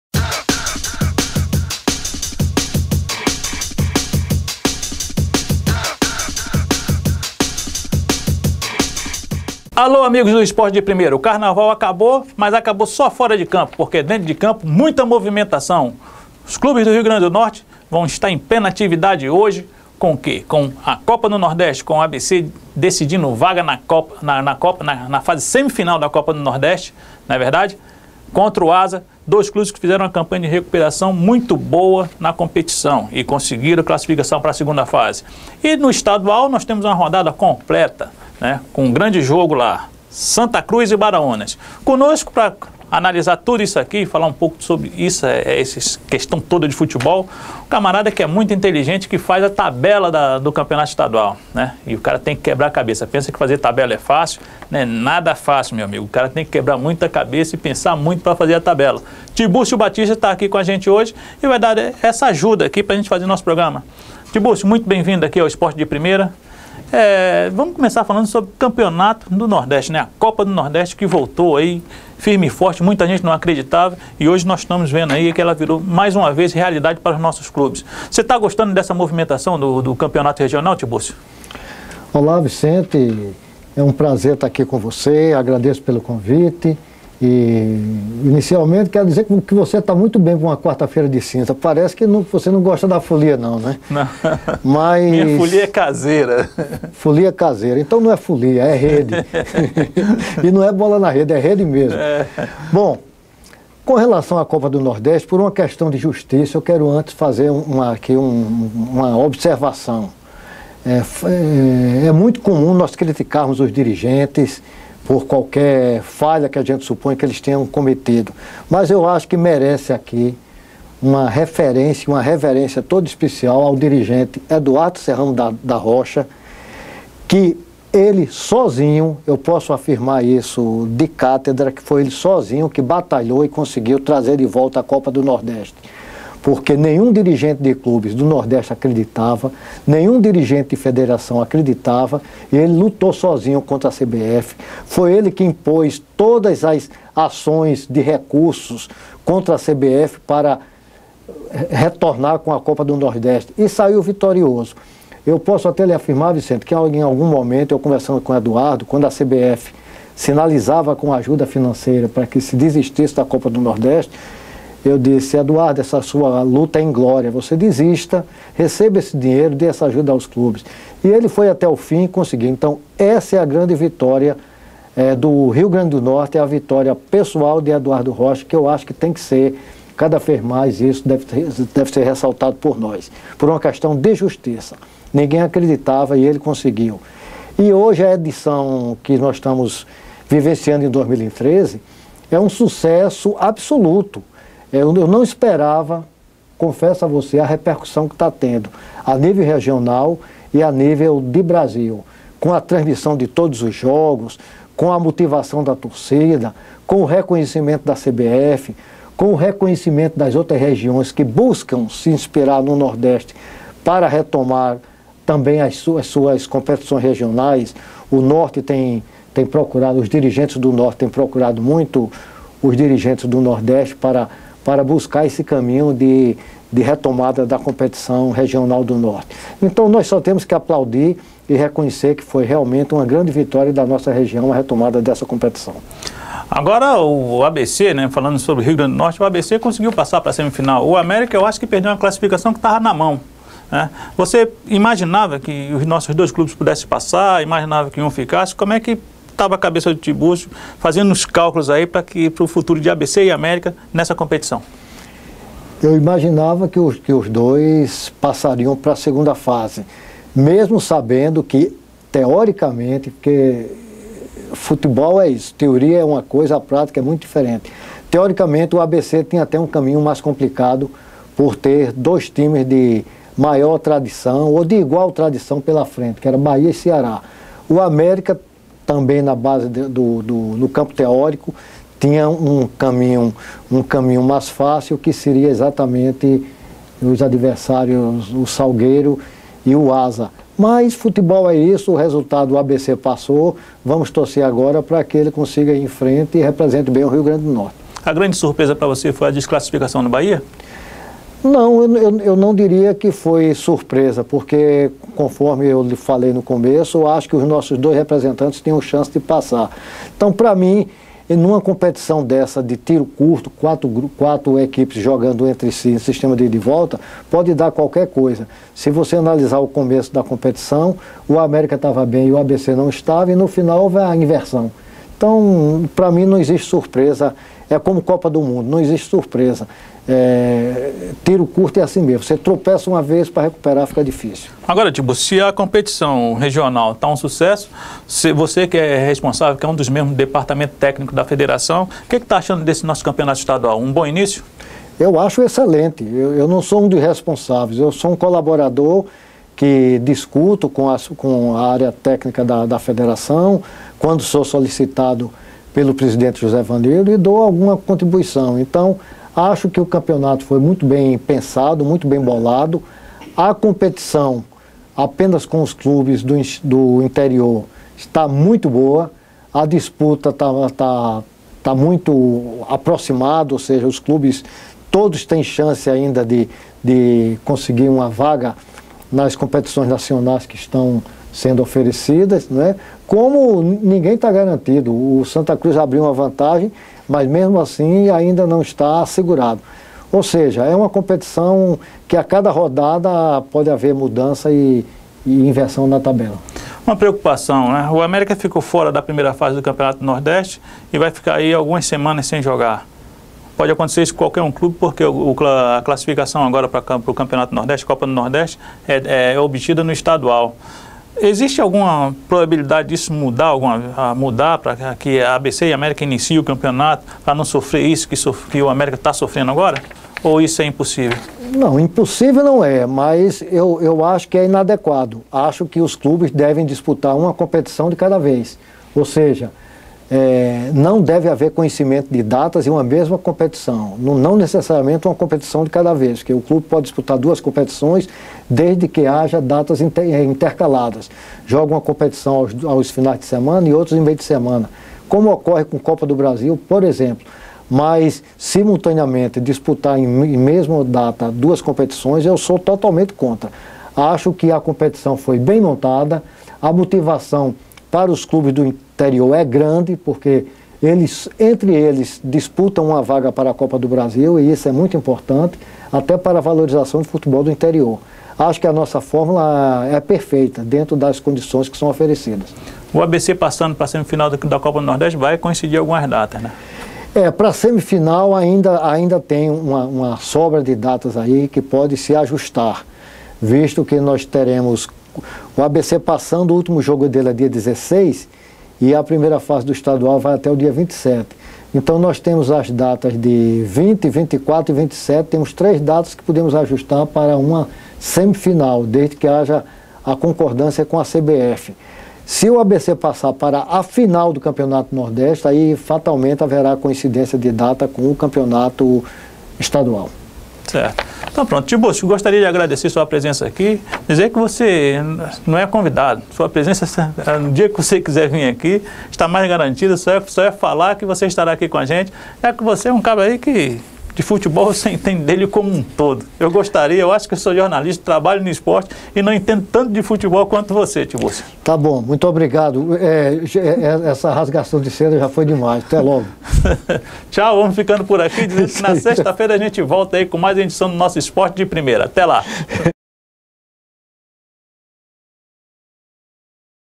[TV TRIBUNA] Federação descarta alteração na forma de disputa do estadual
Integrante do departamento técnico da Federação Norte-riograndense de Futebol, Tibúrcio Batista, acredita no sucesso da Copa Nordeste e acredita que a competição está influindo positivamente até na disputa do Estadual, que serve como qualificação para a disputa do regional de 2014 e ainda assegurou vaga para os clubes de menor investimento do RN na Copa do Brasil.